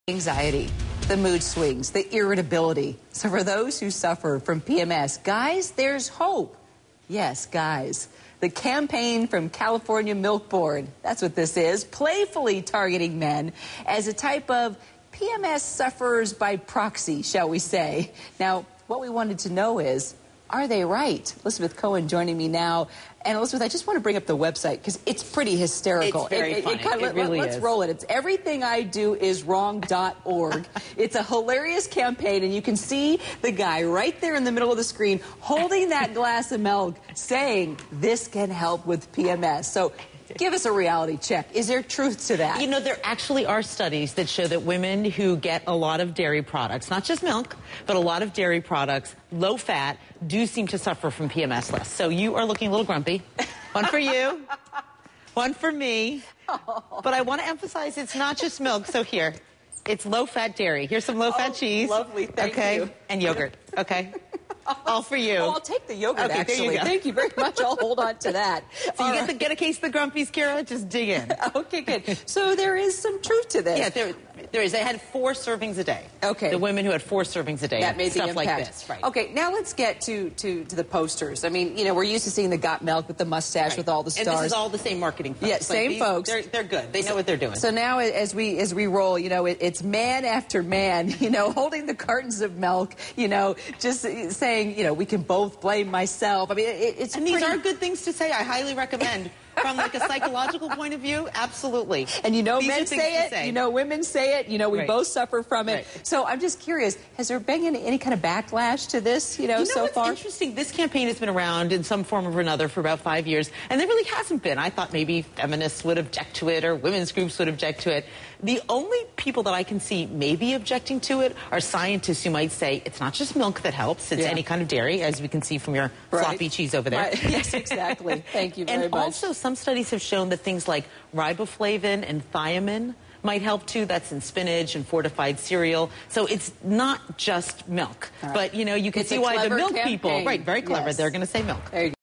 CNN: New ad: Got PMS? Get milk.
A California Milk Board campaign is playfully targeting men by saying milk reduces PMS. CNN's Elizabeth Cohen reports.